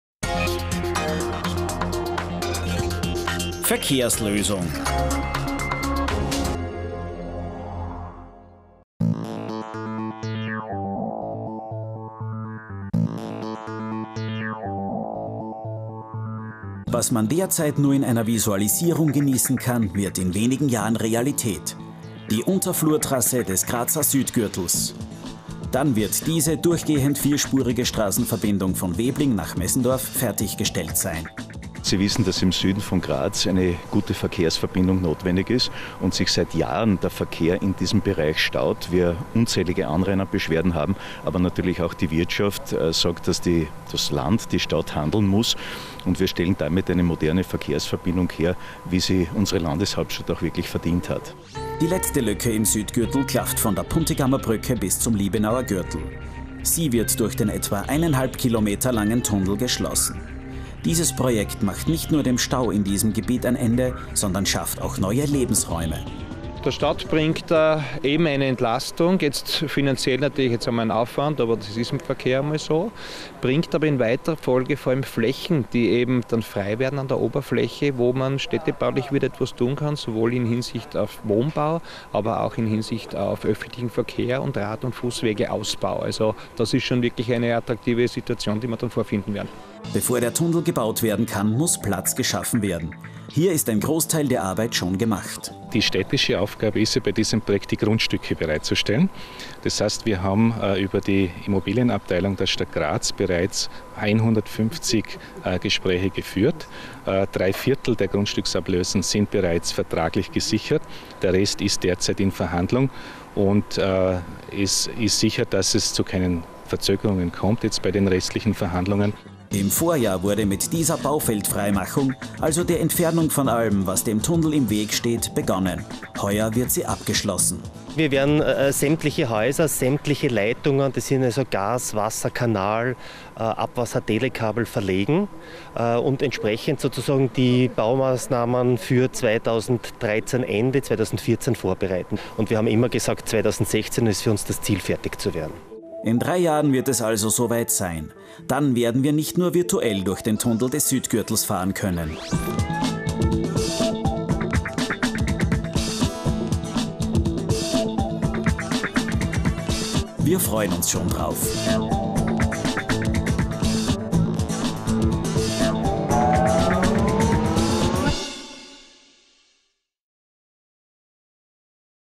Südgürtel Startschuss 2013
Neues vom größten Straßenbauprojekt der Stadt: dem Grazer Südgürtel.